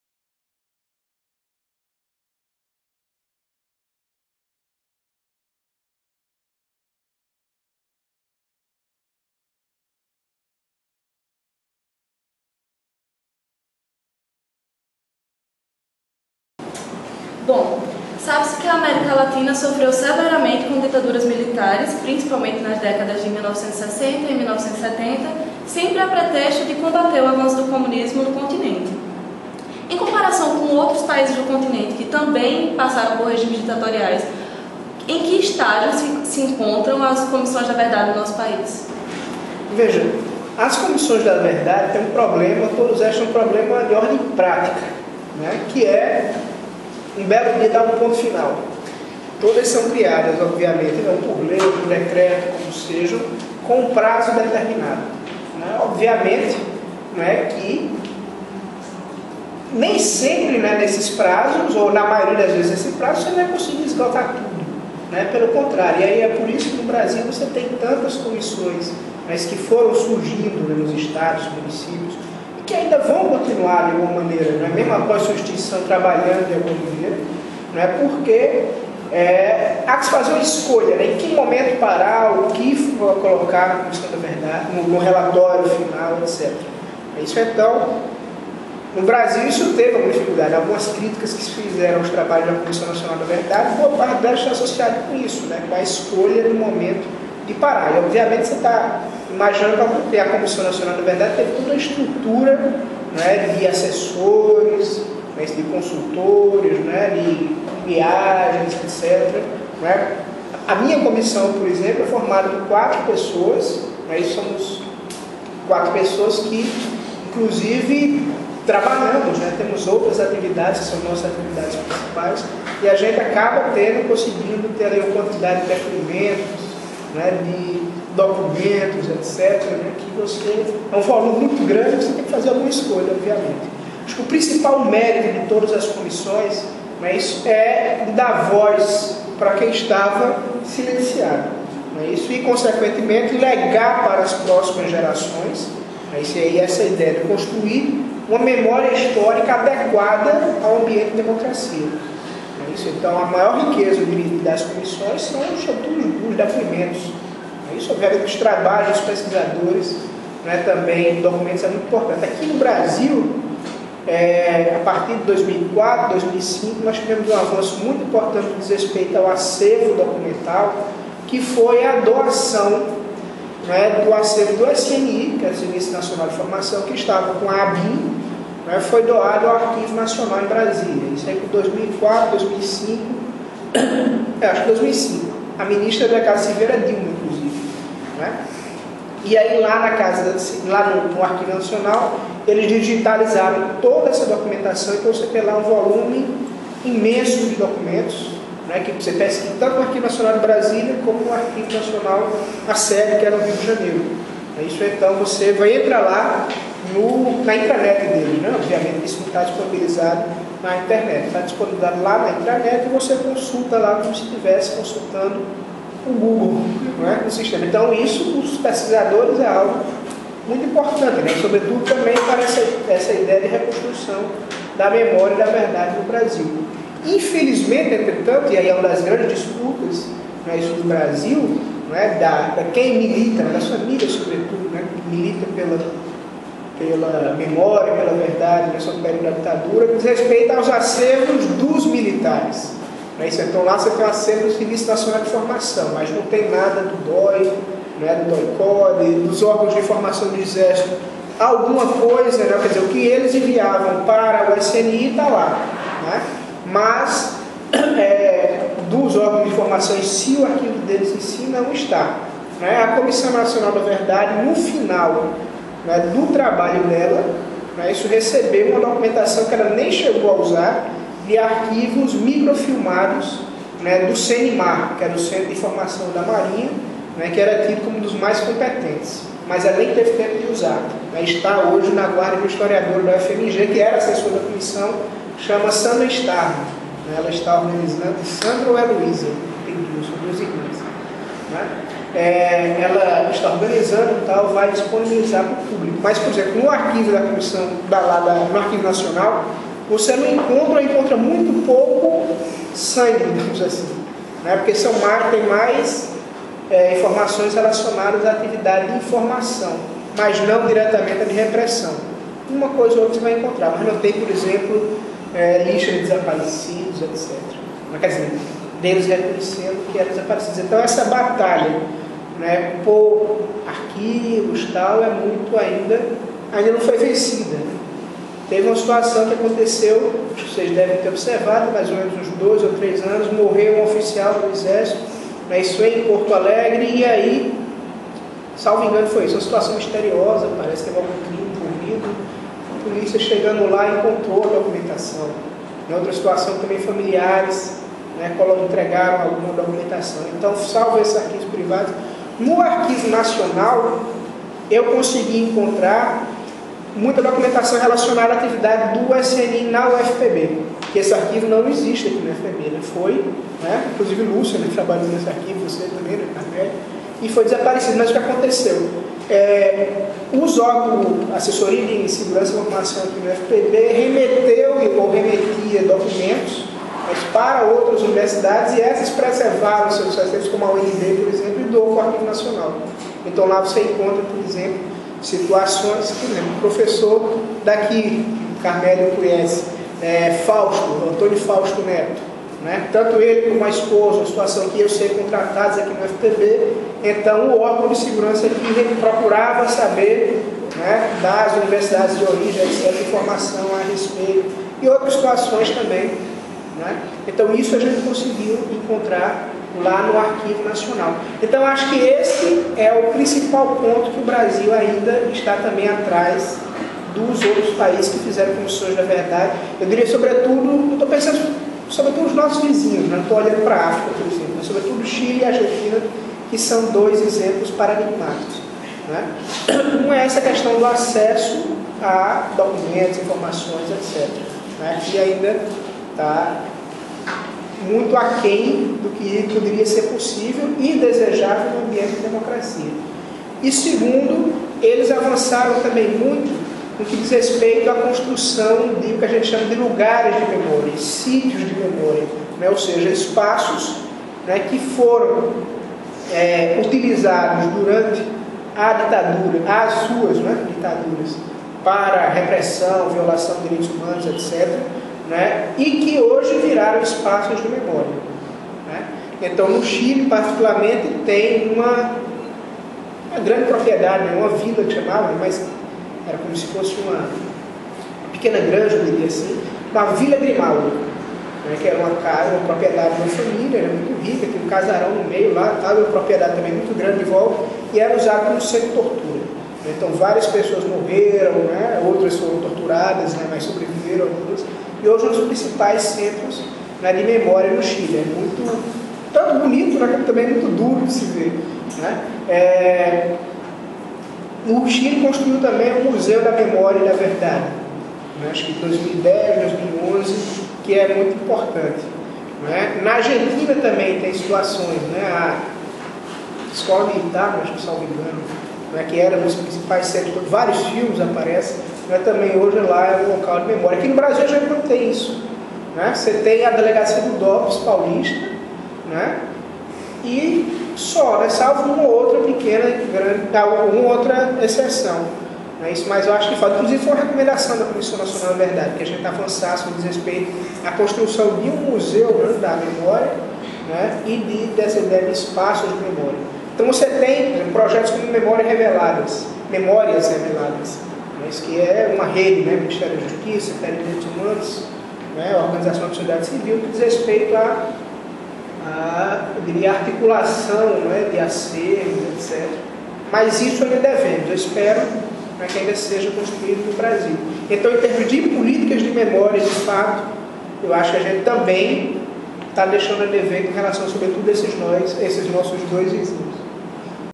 V Colóquio Memória e Verdade, com o professor Rodrigo Freire - 3
3 - Justiça de transição no Brasil e na América Latina
- Neste vídeo, o professor explicita diferenças que caracterizam o modelo de aplicação de justiça de transição no Brasil comparada a países como Argentina e Chile, que também passaram por violações dos direitos humanos durante seus respectivos regimes militares.
Terceiro vídeo do V Colóquio sobre Memória e Verdade, realizado no dia 8 de setembro de 2016 com o professor Rodrigo Freire (Comissão Municipal da Verdade de João Pessoa-PB e Departamento de História da UFPB) e organizado pelas alunas Bianca Patrícia, Gabriela Güllich e Marina Cabral para a disciplina Jornalismo e Cidadania, do curso de Jornalismo da UFPB. Supervisão: Carmélio Reynaldo Ferreira.